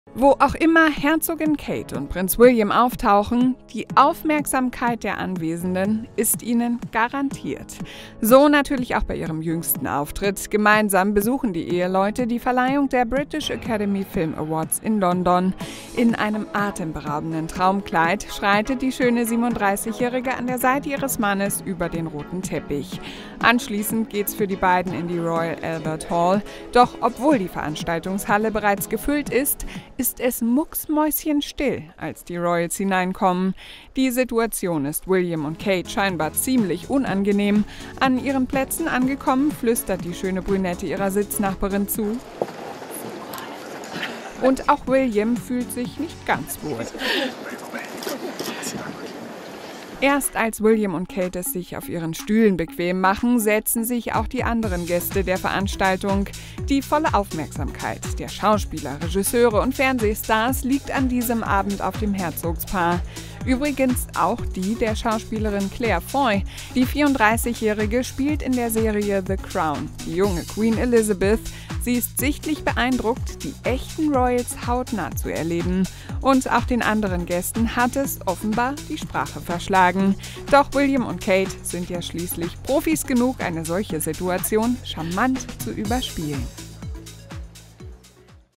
Herzogin Kate - Unangenehme Situation: „Lass uns schnell hinsetzen!“
Wo auch immer Herzogin Kate und Prinz William auftauchen – die Aufmerksamkeit der Anwesenden ist ihnen garantiert. So natürlich auch bei ihrem jüngsten Auftritt.